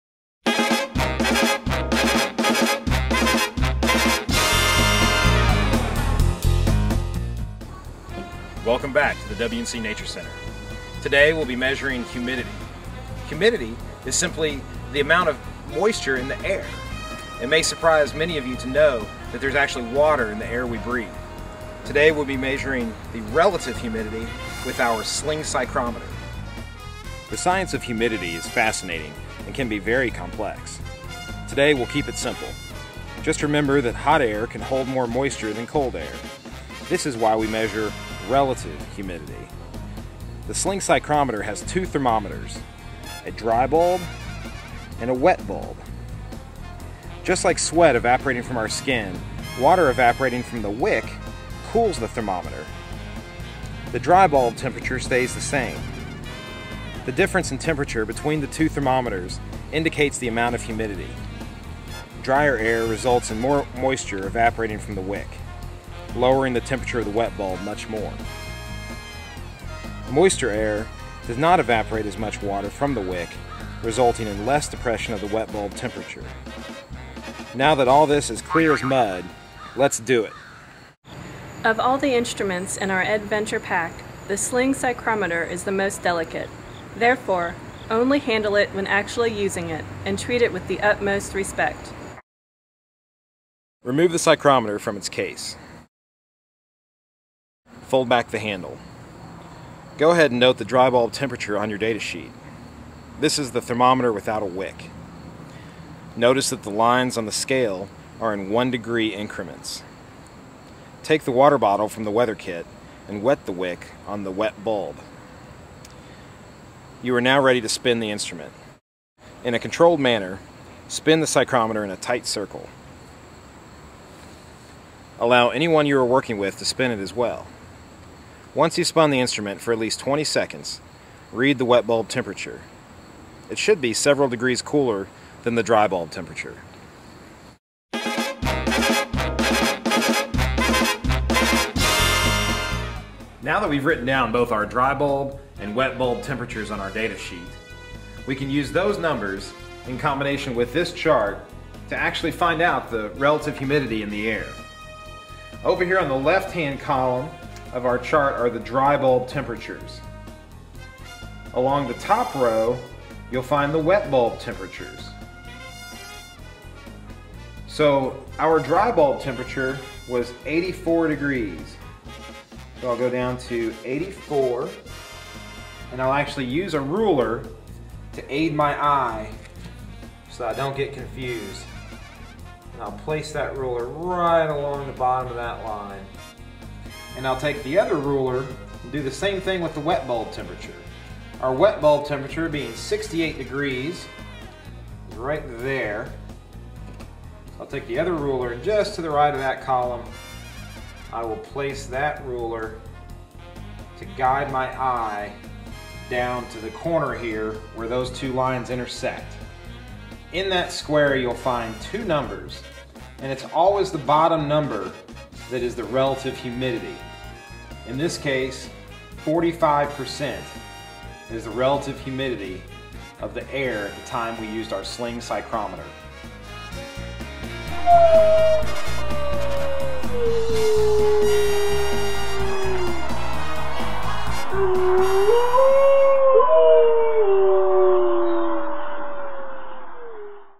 Ed-venture Pack Part 2: Sling Psychrometer
This two-part video series demonstrates for 5th grade students how to use the tools in the WNC Nature Center's Ed-venture Pack to determine various weather conditions at the Nature Center and at their schools.

This second video explains the more complicated concept of relative humidity and demonstrate the process of using the sling psychrometer.